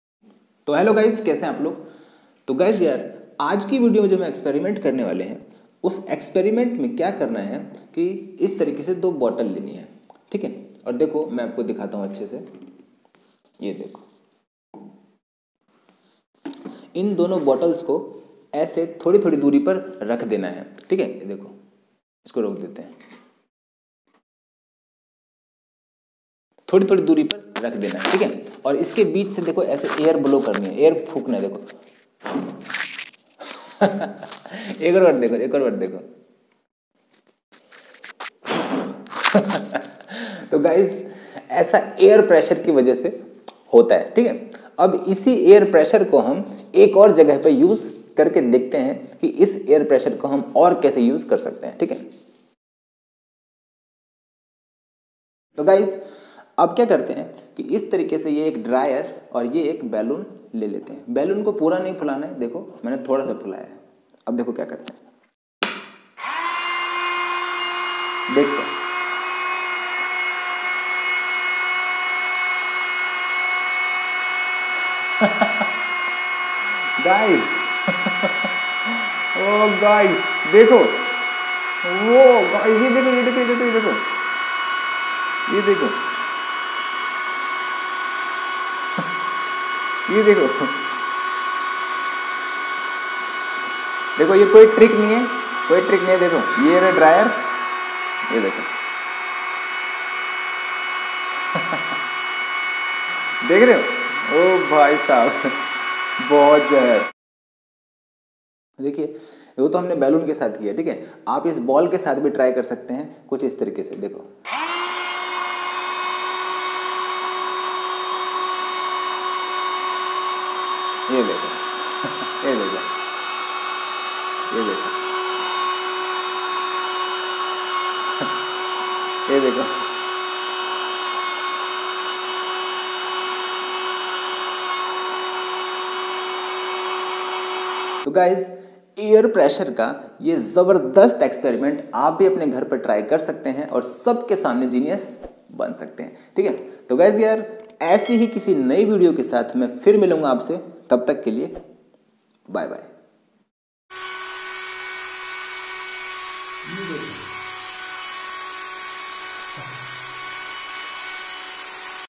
How To Fly A Balloon In Air || गुब्बारे को हवा में कैसे उड़ाएं ? || Easy Experiment At Home
Balloon/Ball Floating in Air || Easy Experiment At Home  || Balloon & Hair Dryer Experiment

Hello guys to kaise hain aap log 🥳

Guys is video me humne Bernoulli Principle pr based ek or experiment kiya hai joki aapko bohot pasand aaega 🙂🙂🙂

 video me kiye experiment ko aap bhi apne ghar pr try kr sakte hain. It's very easy and agar video achhi lgi ho to comment me zarur btayein 🙂🙂

very talented boy Luvkush who made the Hair dryer itself at his home and it works very well

Dancing ball on water video link 👇👇

Thankyou 💕💕
Ambition World
Gulshan